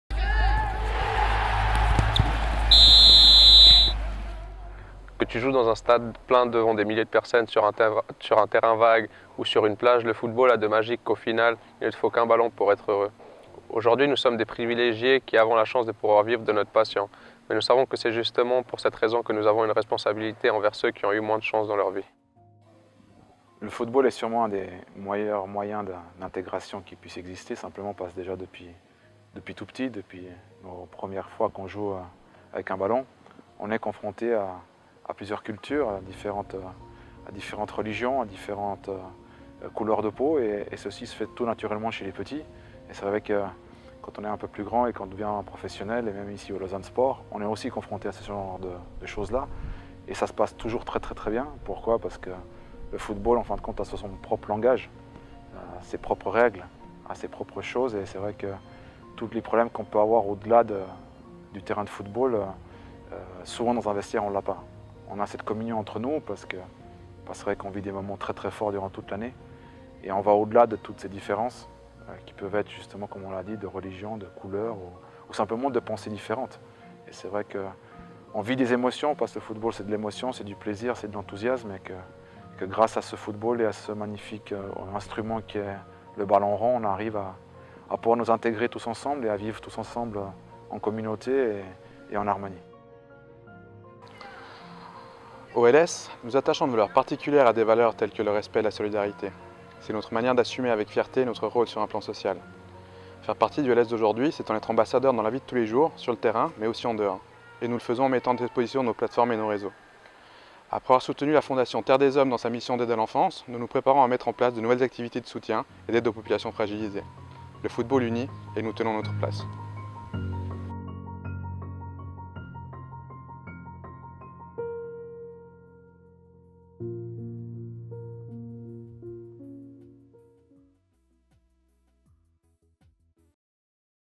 Together 2016 - FC Lausanne (Fr)
Les clubs professionnels suisses mettent l’accent
sur le pouvoir d’intégration du football.